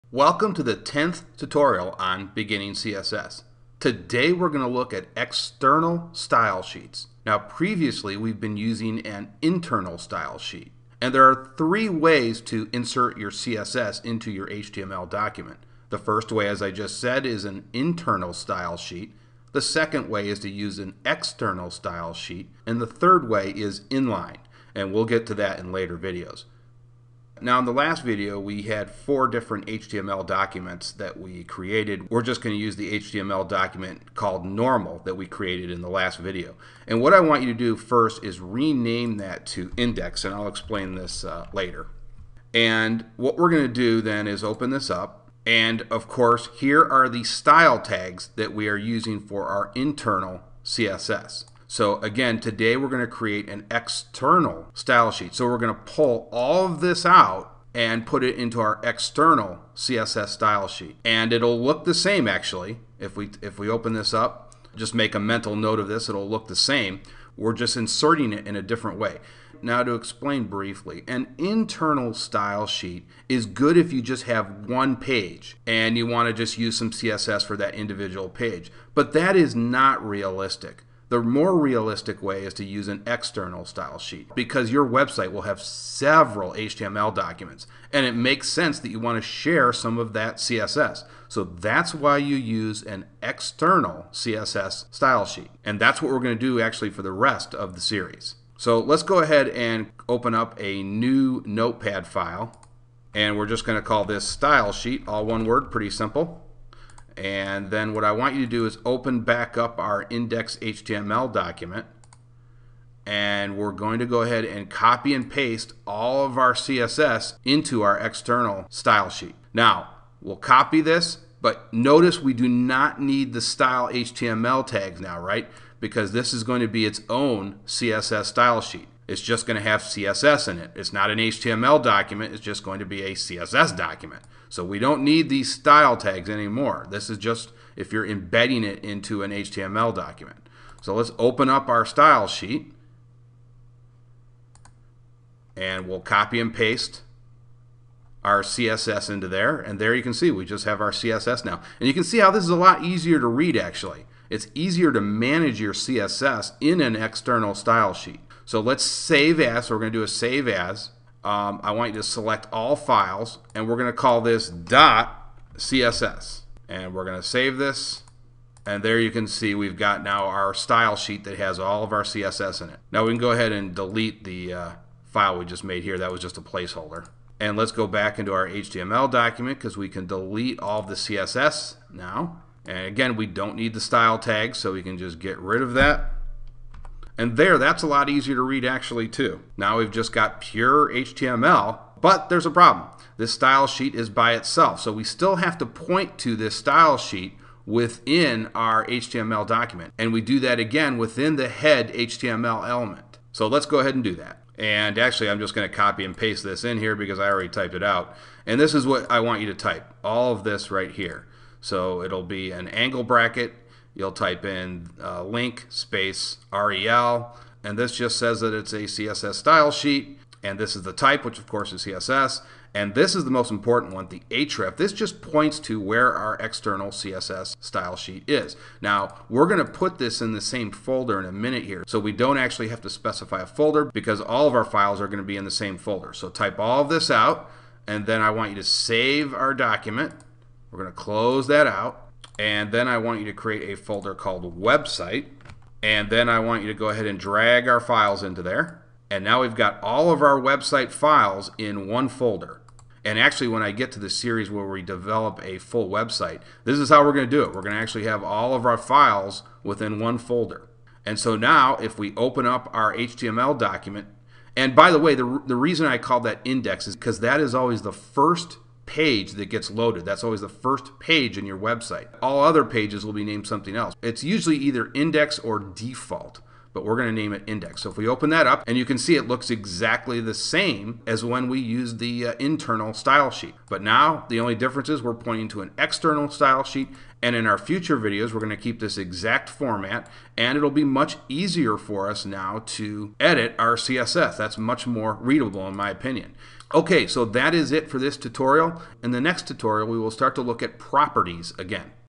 CSS Tutorial for Beginners - 10 - Using an external style sheet
In this video we take a look at the external style sheet.